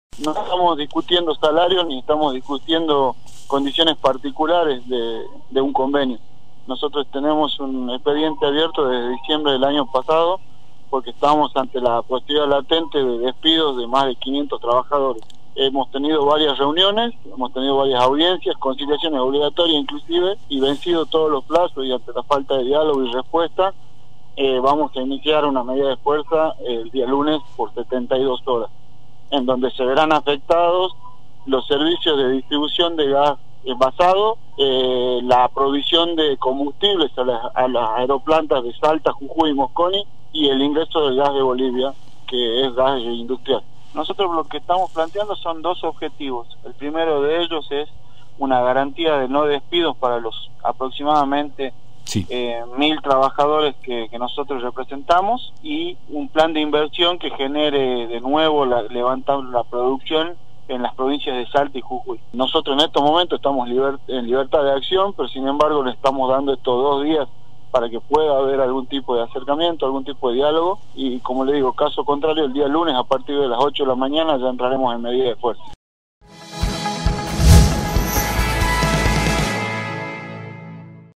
La semana que viene pararán petroleros y no habrá garrafas ni combustible en Salta y Jujuy
La medida de fuerza por 72 horas es porque los trabajadores piden garantías de que no habrá despidos en Refinor.